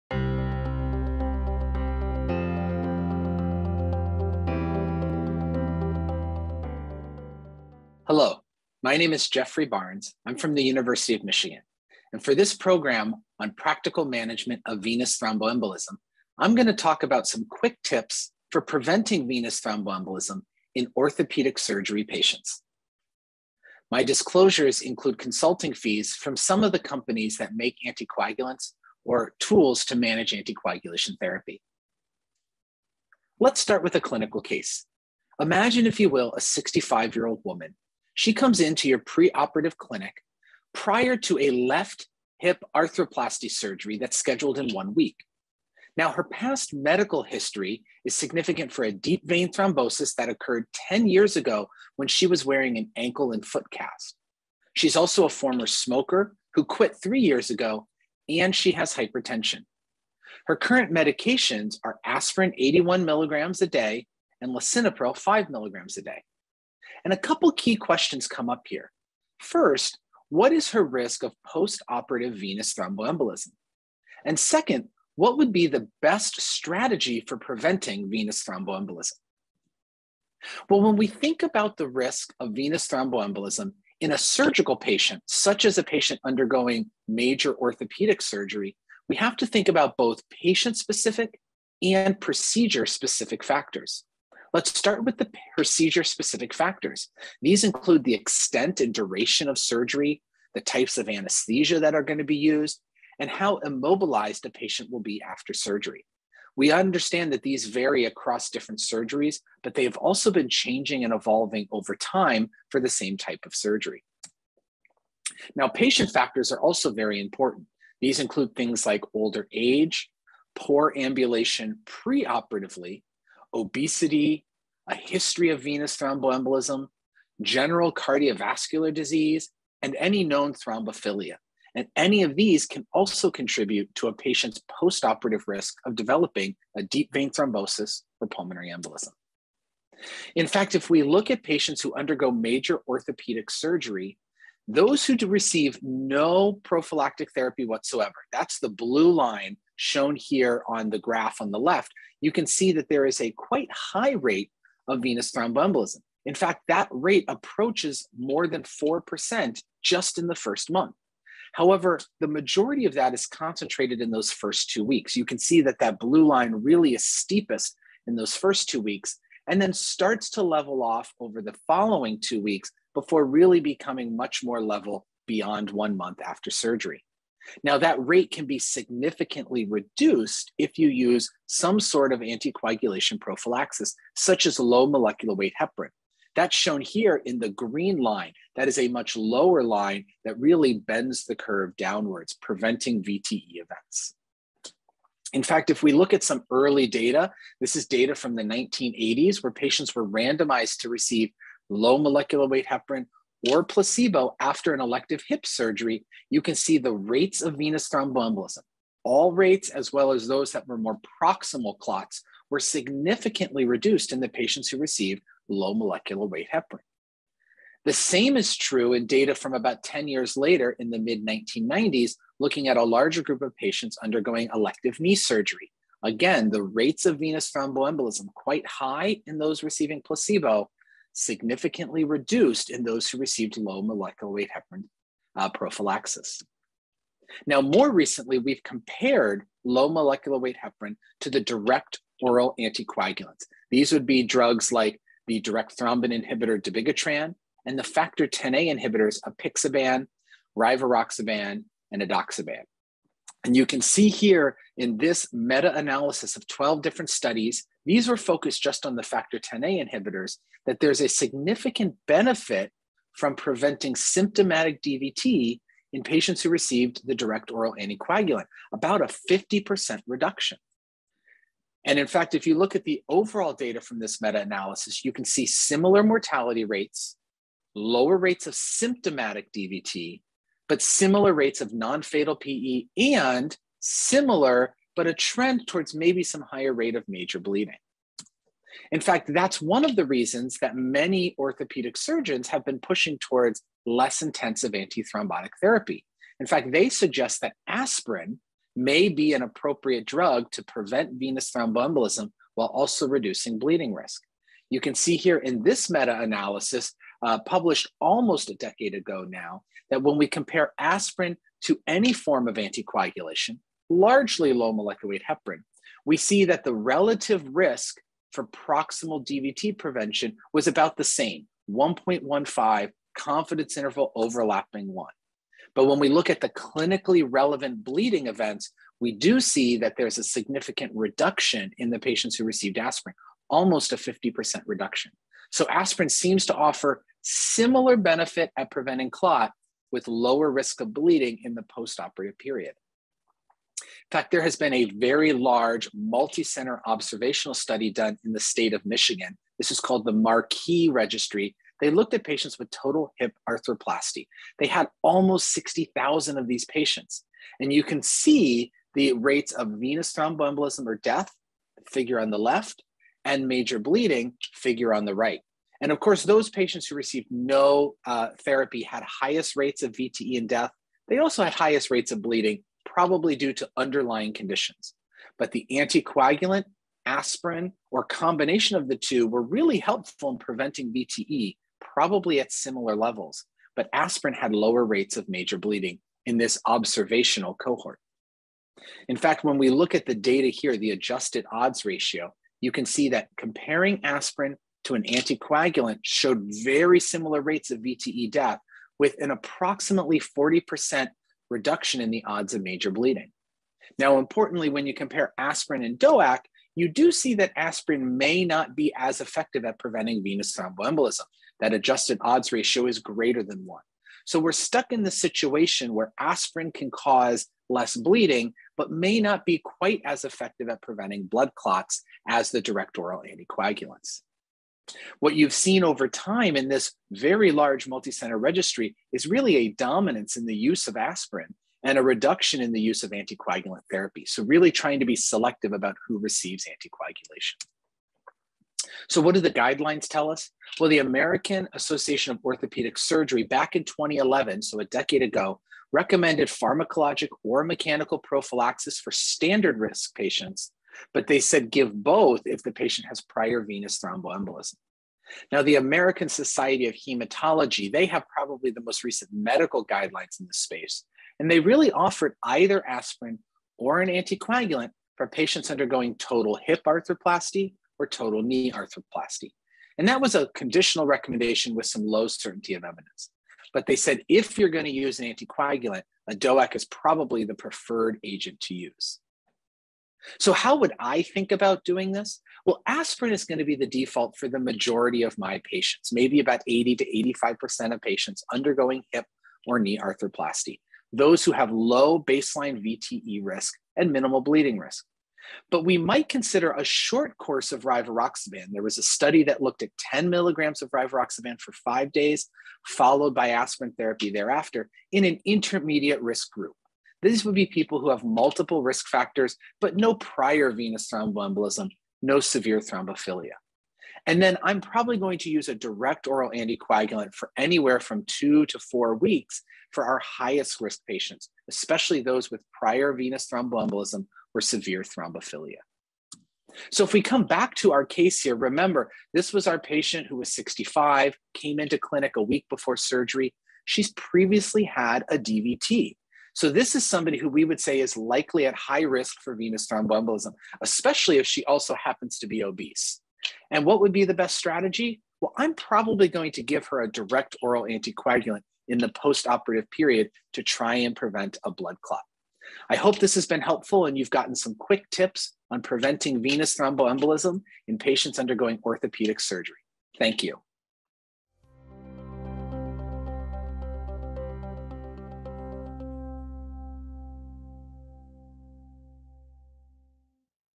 Quick Tips for Preventing VTE in Orthopedic Surgery Patients
In this brief video, Dr. Geoffrey Barnes discusses the risk of VTE following orthopedic surgery, and the best therapy options for low/standard-risk patients compared to high-risk patients. To access more resources on the practical management of VTE,